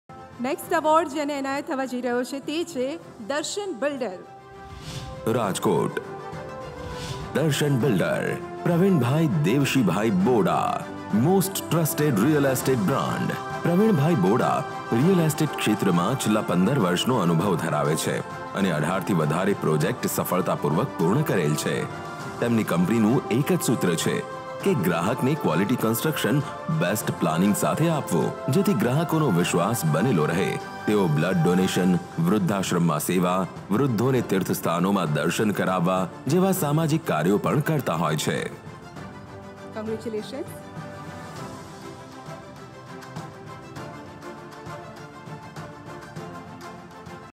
Darshan Group's Big Win: Award from the CM of Gujarat! | Create India Ad
🎉 Exciting news from Create India Advertising! We're super proud to share a special moment: one of our clients in the real estate business just got a prestigious award from the Chief Minister of Gujarat, Mr. Bhupendrabhai Patel. 🏆

🚀 In just 2 years of working with us, they've grown their business 3 times bigger! This award is a big deal for them and for us too. It shows what can happen when we work together and put great ideas into action.